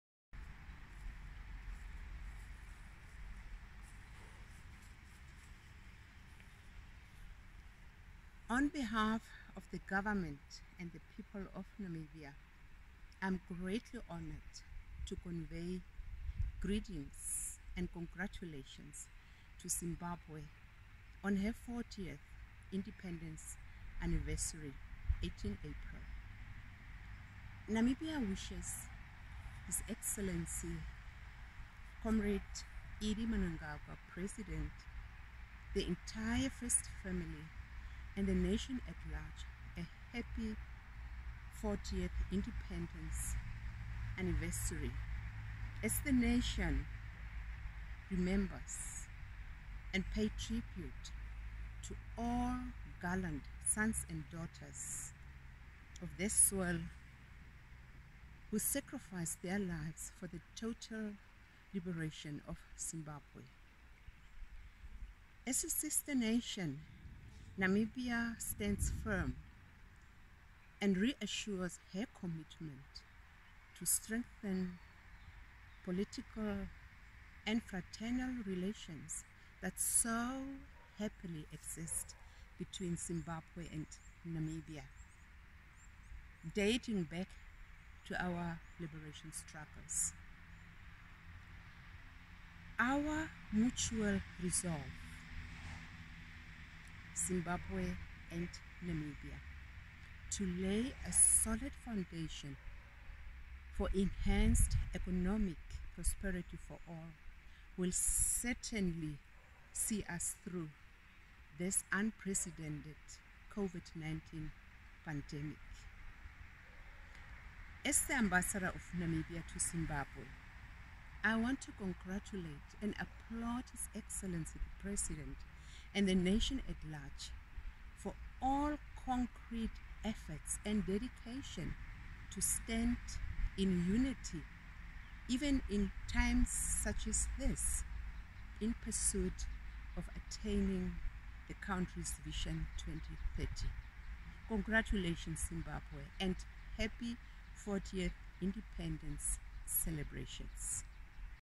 ZIM@40 Namibia Ambassador's message
Namibian Ambassador, Her Excellency Balbina Des Pienaar says her country is committeed to strengthening political relations with Zimbabwe. In her congratulatory message to Zimbabwe on the eve of the country's 40th Independence celebration